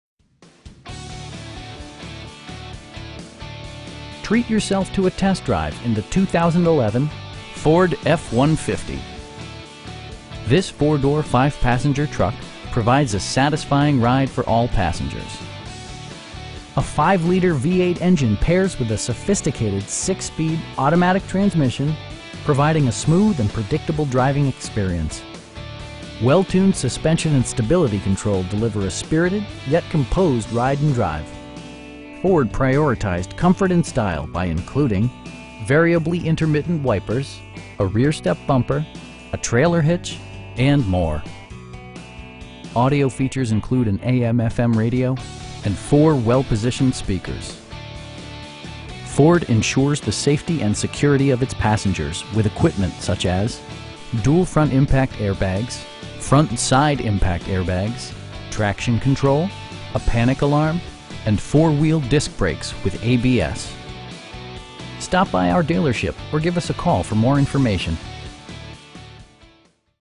2011 Ford F-150 in Irving - Las Colinas, TX 75062
Westway Ford
801 W. Airport Freeway

Treat yourself to a test drive in the 2011 Ford F-150. This 4 door, 5 passenger truck provides a satisfying ride for all passengers. A 5 liter V-8 engine pairs with a sophisticated 6 speed automatic transmission, providing a smooth and predictable driving experience. Well tuned suspension and stability control deliver a spirited, yet composed, ride and drive Ford prioritized comfort and style by including: variably intermittent wipers, a rear step bumper, front bucket seats, air conditioning, an overhead console, a trailer hitch, and more. Audio features include an AM/FM radio, and 4 speakers, providing excellent sound throughout the cabin. Ford ensures the safety and security of its passengers with equipment such as: dual front impact airbags, front SIDE impact airbags, traction control, a panic alarm, and 4 wheel disc brakes with ABS. We offer competitive pricing, and the area's best shopping experience. Stop by our dealership or give us a call for more information.